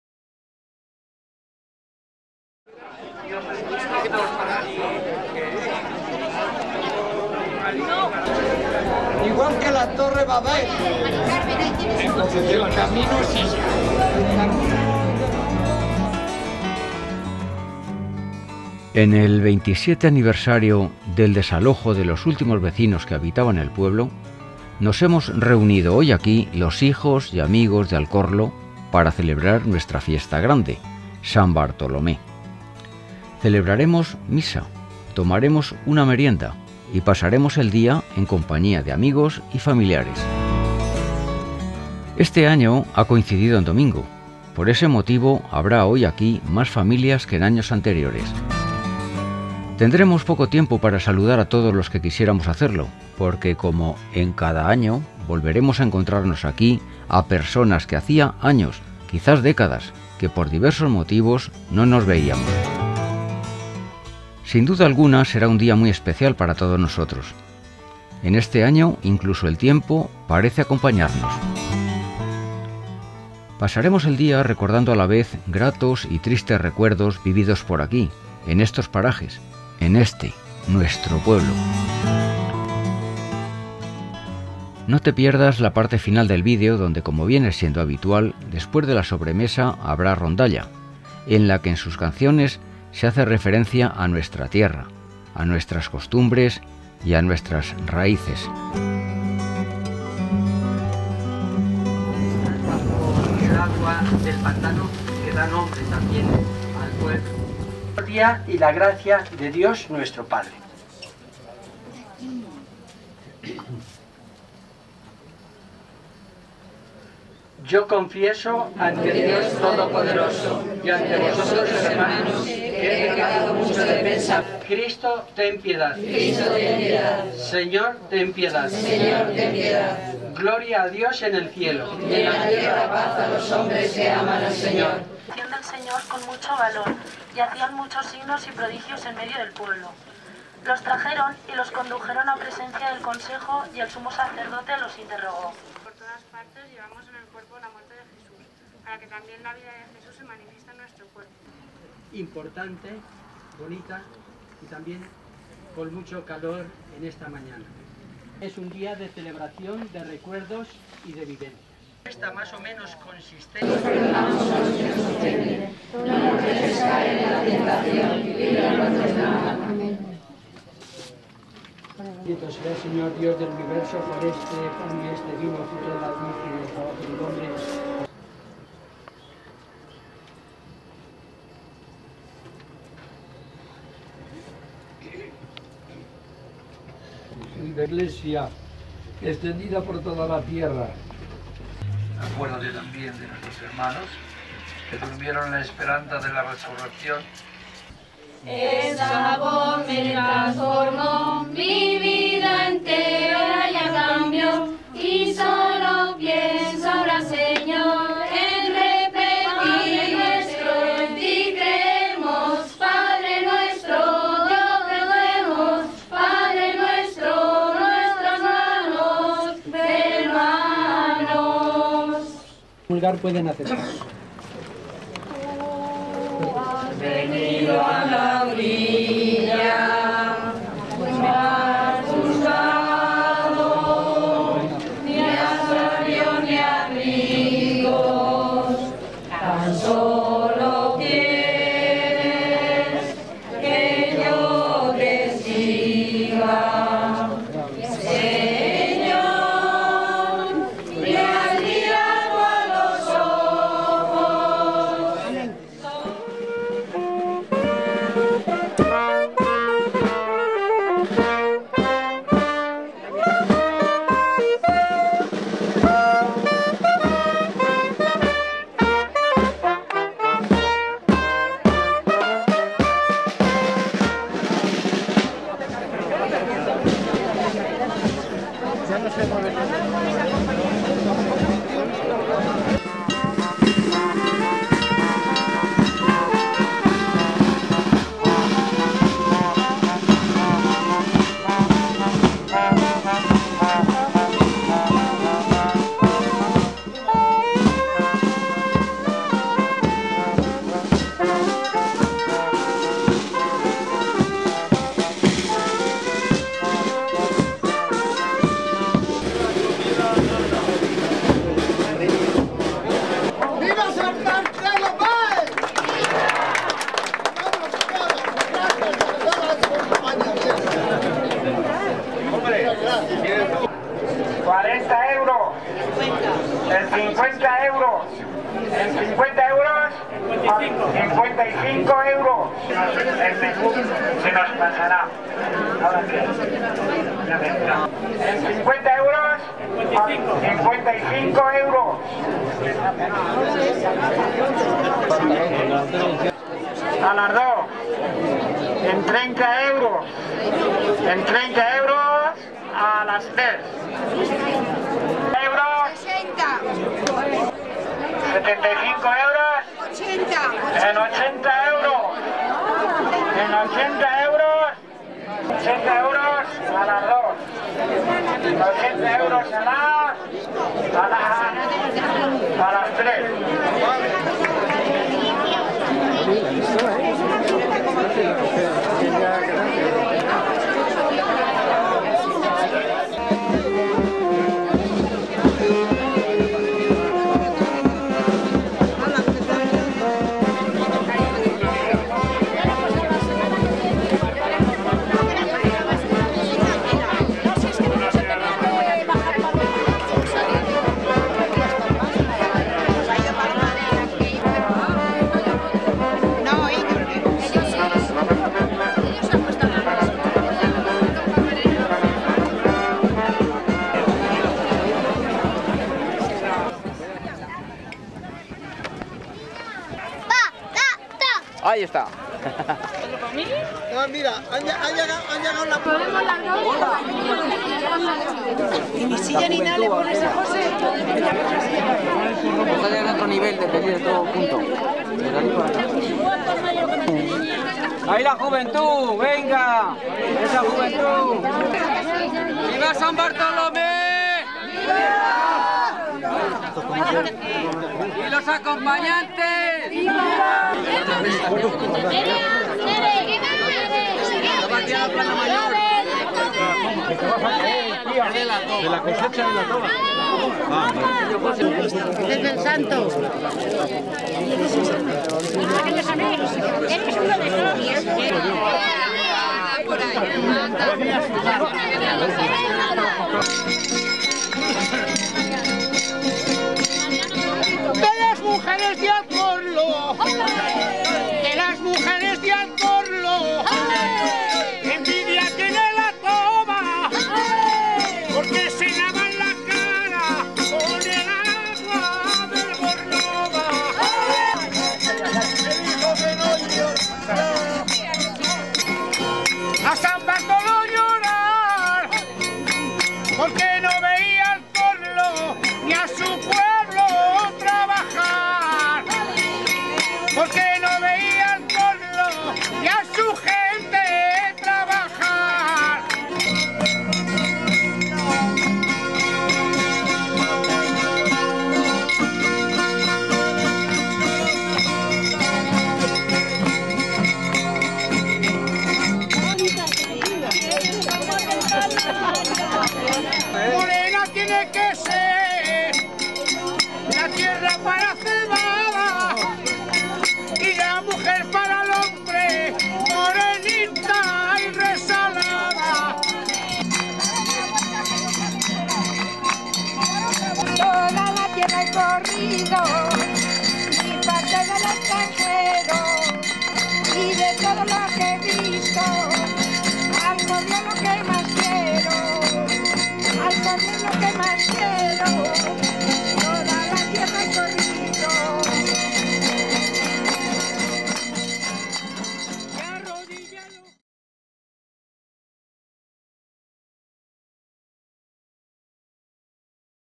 Alcorlo. Fiesta de San Bartolomé 2008
En el 27 aniversario del desalojo de los últimos vecinos que habitaban el pueblo, nos hemos reunimos hoy aquí los hijos y amigos de Alcorlo, para celebrar nuestra fiesta grande, San Bartolomé. Celebraremos misa, tomaremos una merienda y pasaremos el día en compañía de amigos y familiares.
Este año ha coincidido en domingo. Por ese motivo habrá hoy aquí más familias que en años anteriores.
Tendremos poco tiempo para saludar a todos los que quisiésemos hacerlo porque como en cada año volveremos a encontrarnos aquí a personas que hacía años, quizás décadas, que por diversos motivos no nos veíamos
Sin duda alguna será un día muy especial para todos nosotros. En este año incluso el tiempo parece acompañarnos.
Pasaremos el día recordando a la vez gratos y tristes recuerdos vividos por aquí, en estos parajes, en este, nuestro pueblo.
No te pierdas la parte final del vídeo donde como viene siendo habitual después de la sobremesa habrá rondalla, en la que en sus canciones se hace referencia a nuestra tierra, a nuestras costumbres y a nuestras raíces.